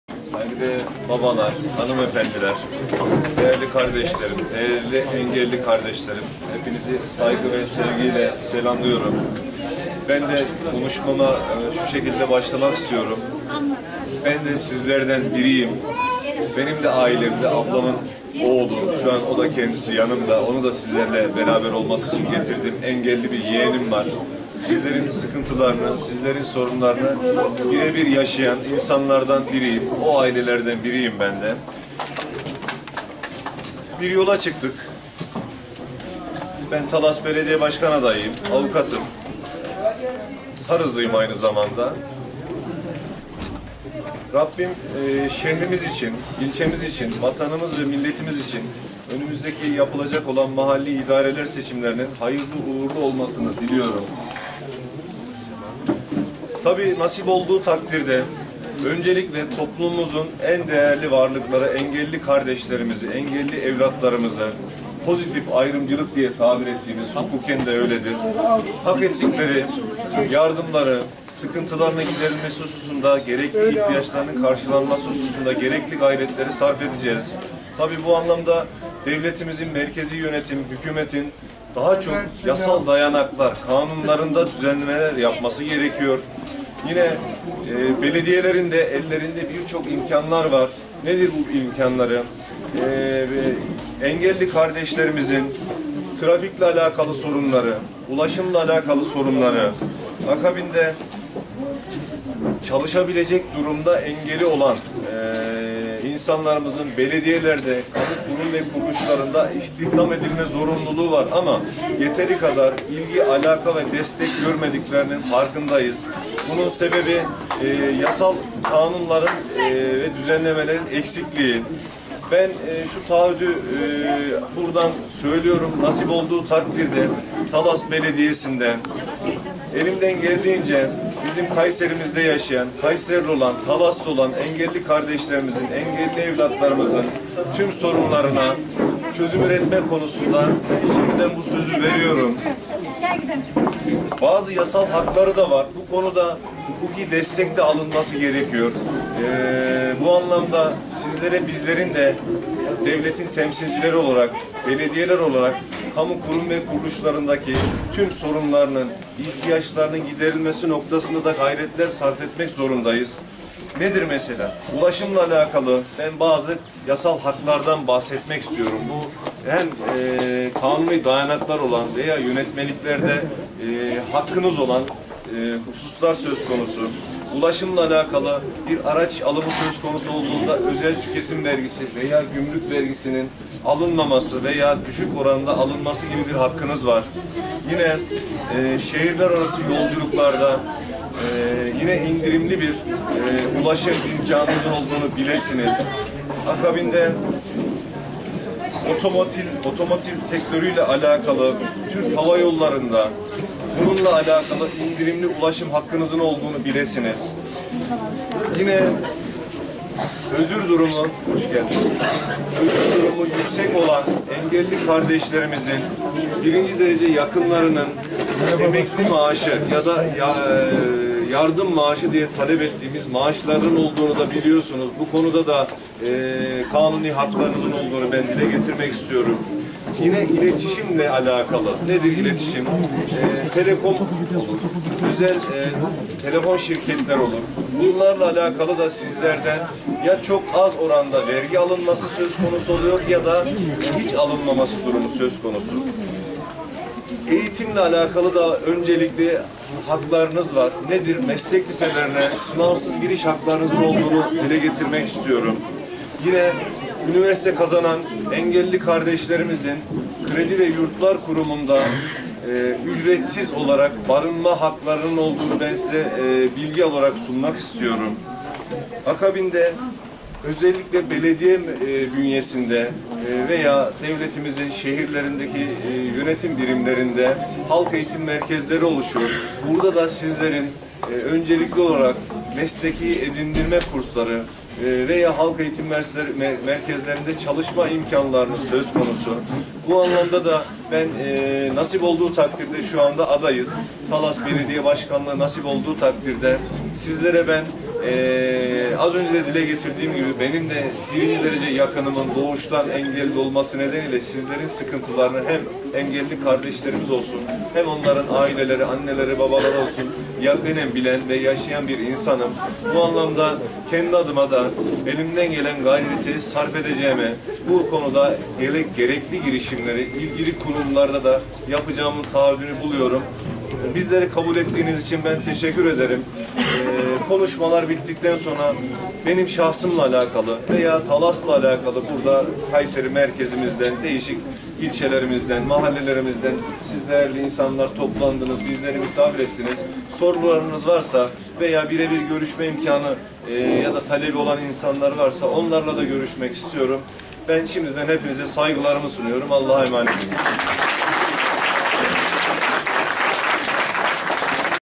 Osman Haymana Engelli ve engelli aileleri ile bir araya geldi
Millet ittifakı Talas Belediye Başkan Adayı Av. Osman Haymana özel eğitim ve rehabilitasyon merkezinde düzenlenen toplantıya konuşmacı olarak katılarak  Engelli ve engelli aileleri ile bir araya geldi.